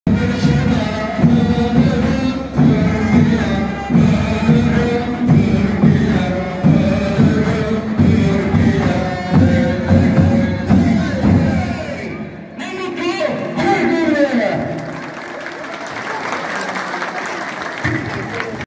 Yiğit Özkul sünnet düğünü. 18 Haziran 2023 - 6
Menteşe İlk/Orta okulu Müdürü İbrahim Özkul ile Emre Özkul'un oğlu Yiğit Özkul 'un sünnet merasimi ve düğünü. 18 Haziran 2023